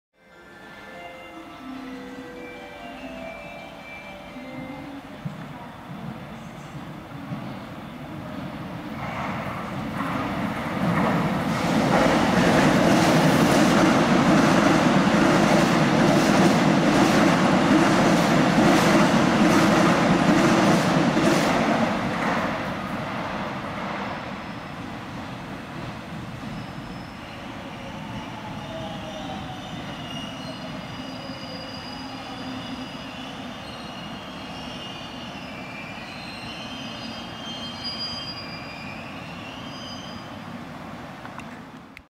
[철도영상] 코레일 경인선 오류동역에서 스쳐가는 두 열차 - K175열차(인천행) / K1114열차(용산급행) (2017.10.21)
[FHD 60fps 지원] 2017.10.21 수도권전철 1호선 오류동역

(정차) 일반전동열차 제K175열차
양주 → 인천
※311x04편성

(통과) 급행전동열차 제K1114열차
동인천 → 용산